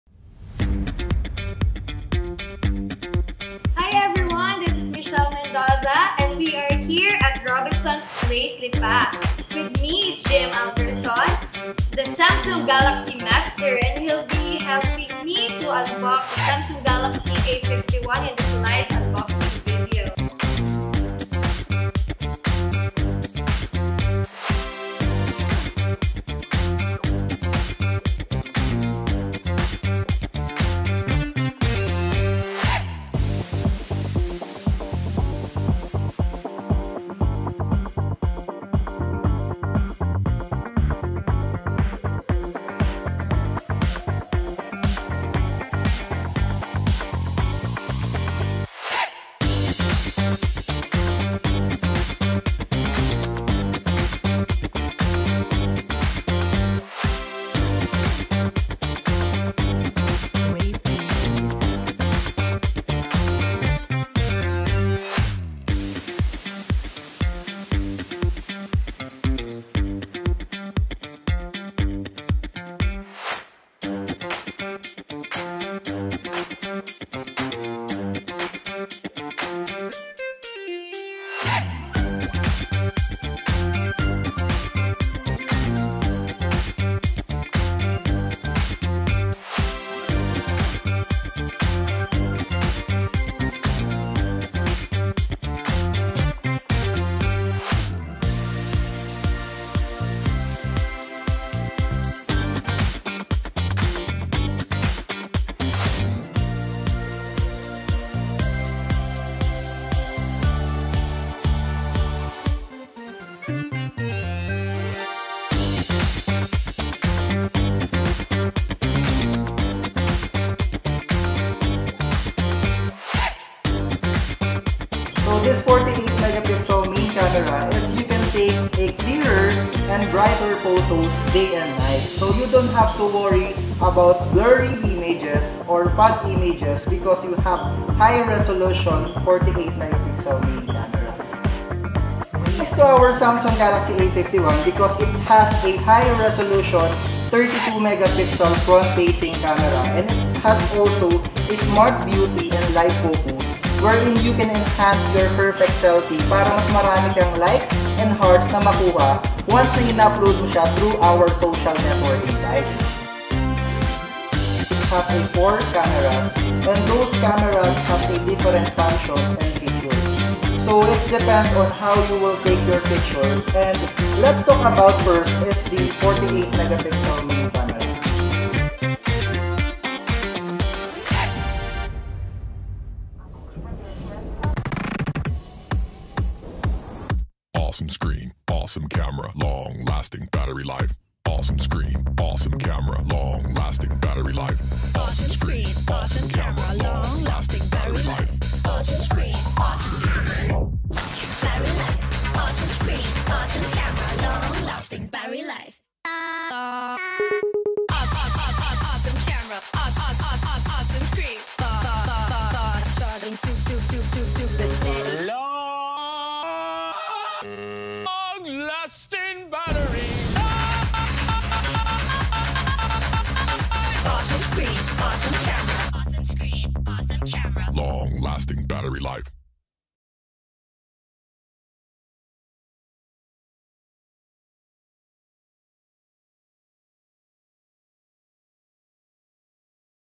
Samsung Galaxy A51 Launching at Robinson's Place Lipa
Get an awesome screen, an awesome camera, and a long-lasting battery with the Galaxy A51. Available online and in-store.